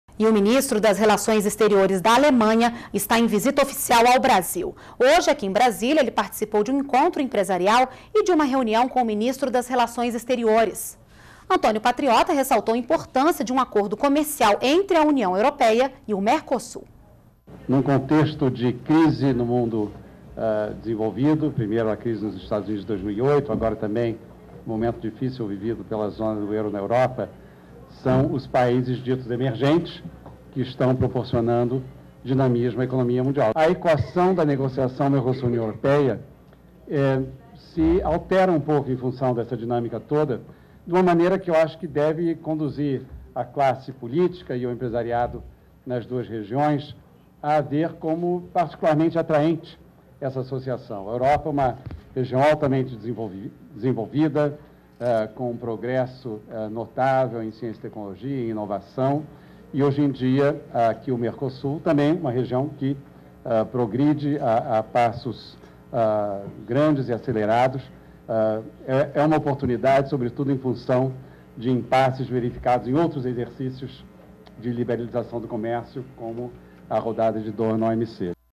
Ministro Antonio Patriota destaca acordo comercial entre União Europeia e Mercasul
NBR Notícias - 13.02.12: O ministro das Relações Exteriores da Alemanha, Guido Westerwelle, esteve em visita oficial ao Brasil. Nesta segunda-feira (13), em Brasília, participou de um encontro empresarial e de uma reunião com ministro das Relações Exteriores, Antônio Patriota. O ministro Antônio Patriota ressaltou a importância de um acordo comercial entre a União Europeia e o Mercosul.